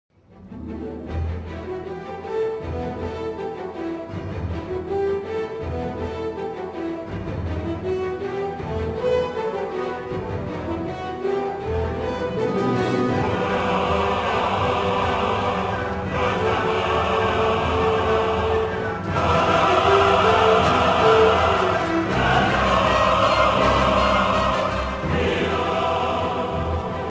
Obiwan vs Darth Maul practice attempt Part 3!
Here it is everyone our last attempt with a little of proper background music to get it right 😉